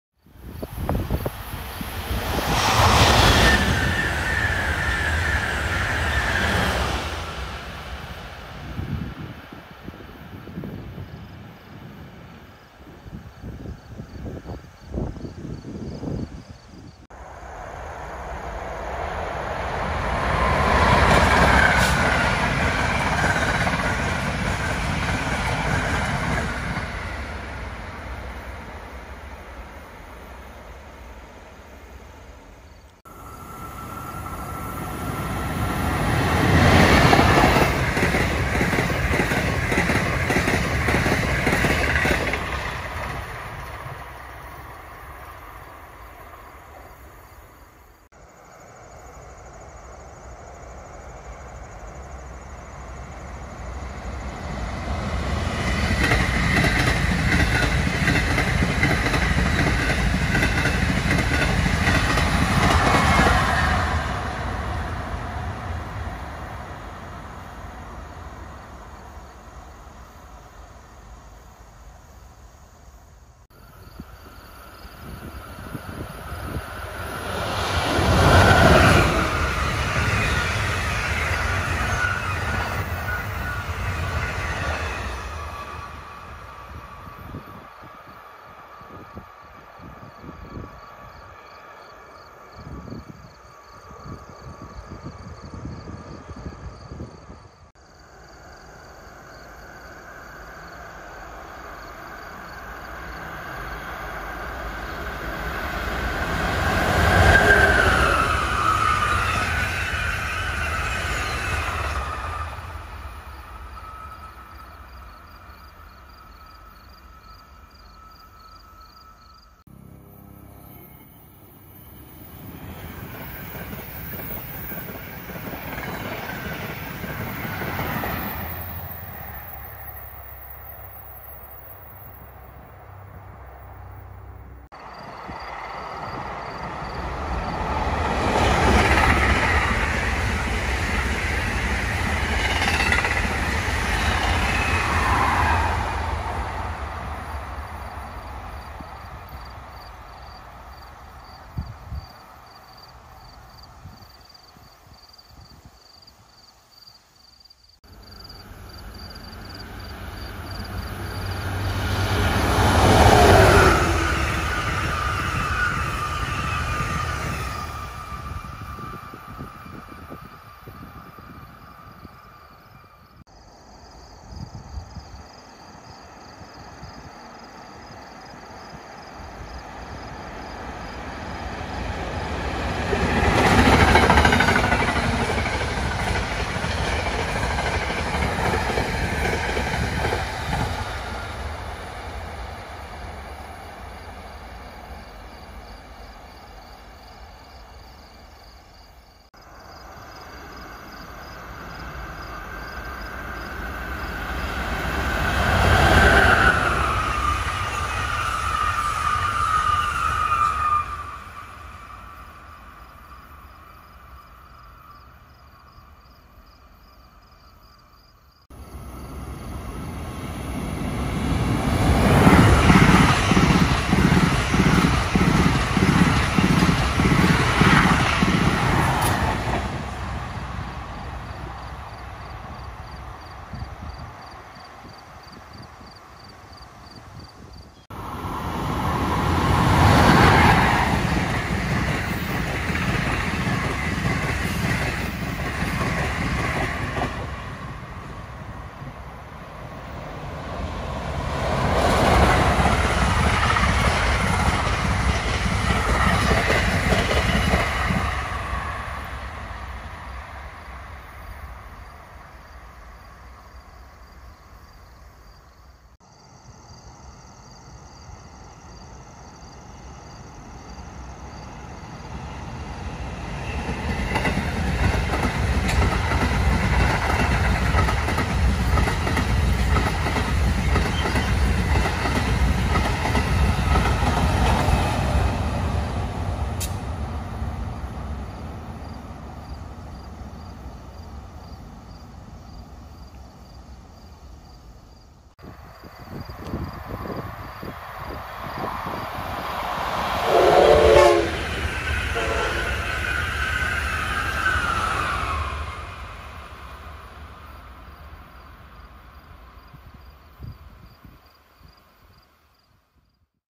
Amtrak & NJ Transit Northeast Corridor Racetrack Afternoon Action @ Deans NJ (September 2020)
We open and close with Acela Express trains, and NJT PRR Heritage Unit 4636 makes an appearance. Enjoy =)